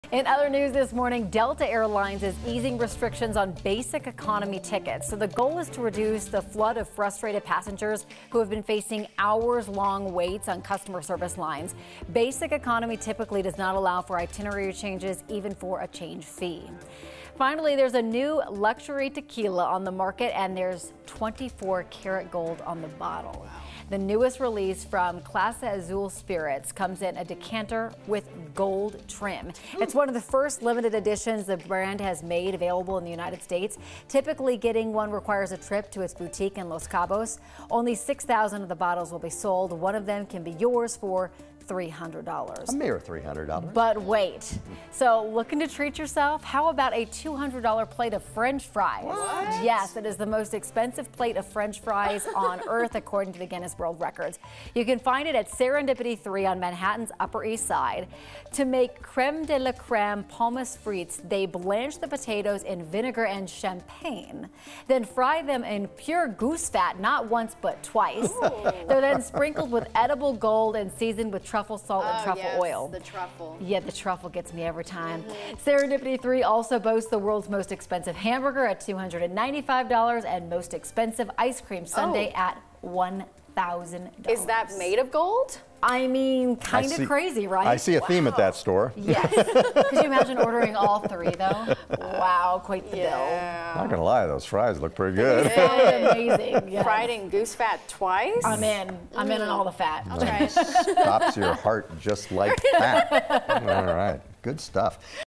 In Other News: Delta easing restrictions on basic economy tickets, luxury tequila bottle made with 2
Corey Rose shares three stories to tell your friends about on the morning of August 2, 2021.

9NEWS (KUSA) is located in Denver, Colorado.